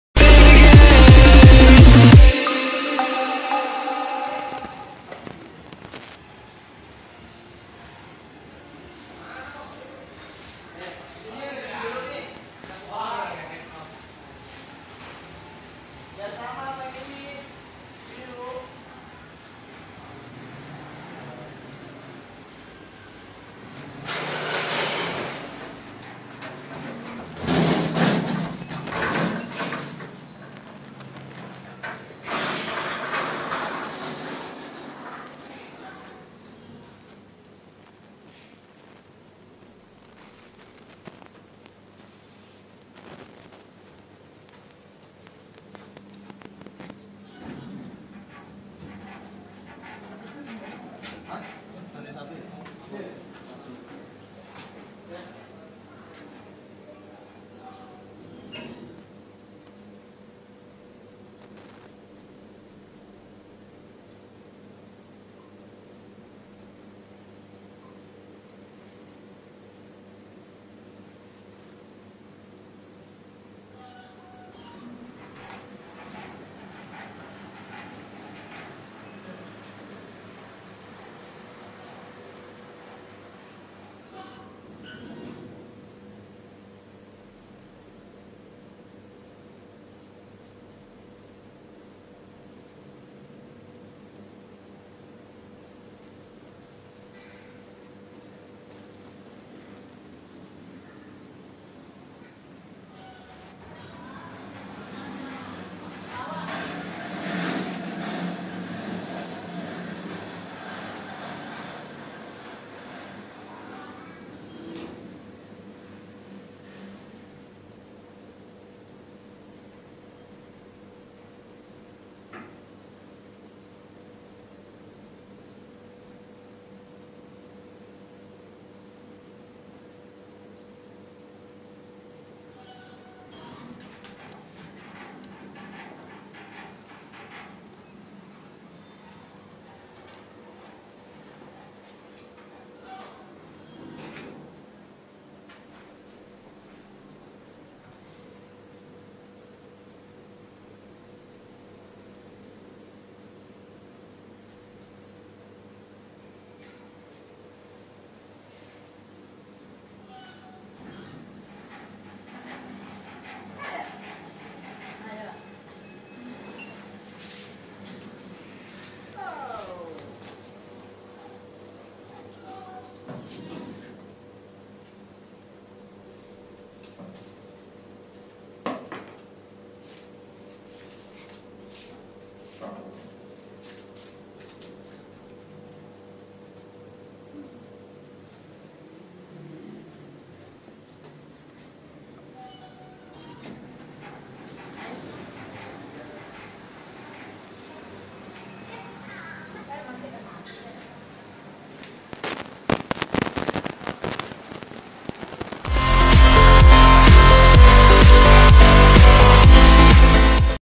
Grotty Death Trap Generic Lift at Pasar Senen Jaya Blok 5, Jakarta
This is another grotty lift with a totally generic fixtures, which is like one of the Louser lifts in RSPP (Pertamina Central Hospital). Enjoy!

BUILDING DATA
Name: Pasar Senen Jaya Blok 5
Location: Jl. Senen Raya, Senen, Jakarta, Indonesia

LIFT DATA
Capacity: Unknown
Floors served: 1, 2*, 3 - 7
History: Refurbished
(*) Locked floor

Camera: Evercoss A74A
Edited with: Vegas Pro 14
Taken date: November 19th, 2016